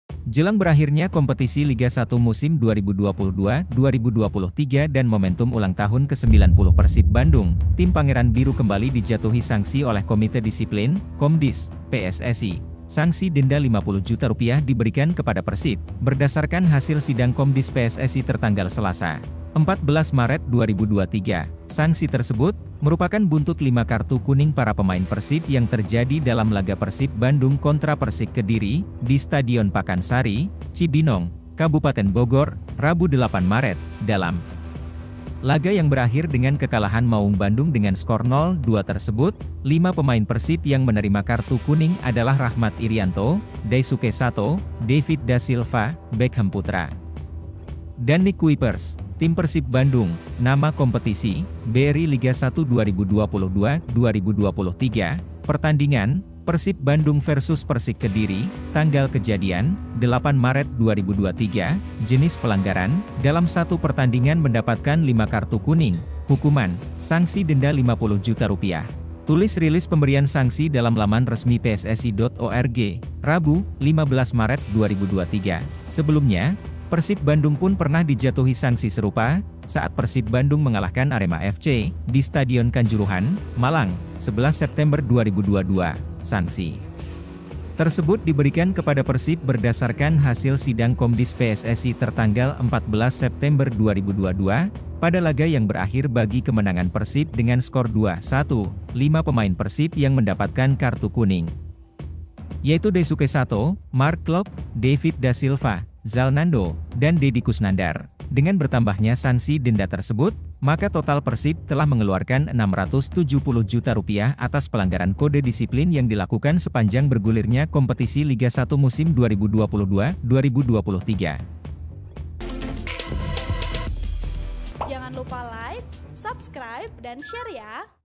Persib Bandung Kembali Disanksi Rp 50 Juta, Total Maung Sudah Didenda Setengah Miliar Lebih
Berita selengkapnya, klik tautan berikut ini:
Penulis: Cipta Permana | Editor: Januar Pribadi Hamel |Video Editor: Uben